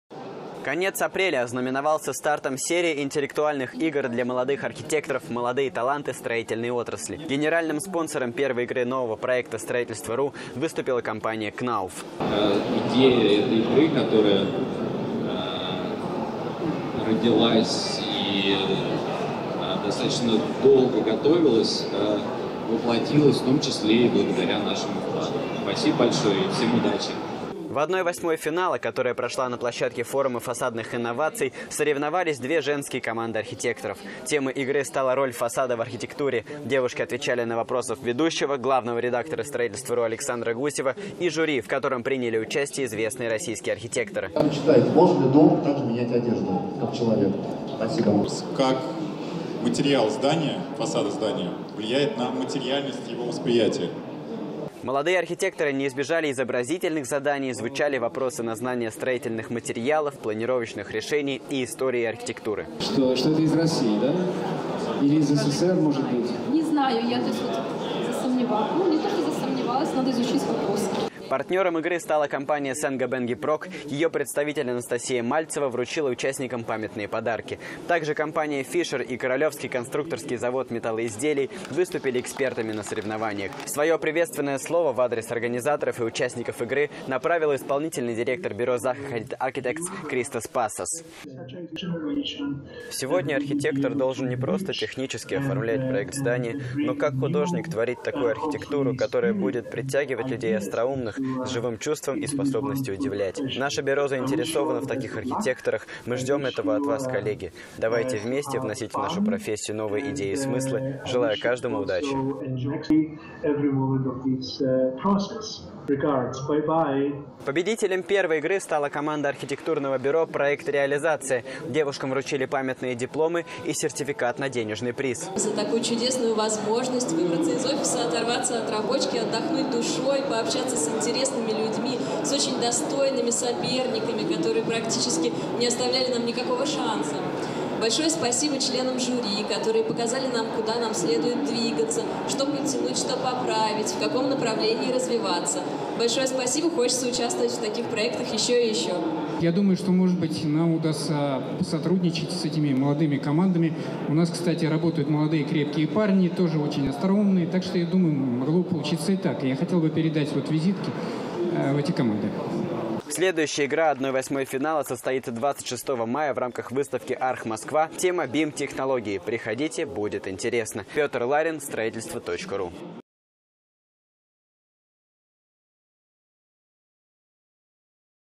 Как прошла первая в России интеллектуальная игра для архитекторов
"Строительство.RU" запускает проект "Молодые таланты строительной отрасли" - интеллектуальную игру-соревнование для молодых архитекторов. Об особенностях новой дискуссионной площадки - в репортаже Петра Ларина.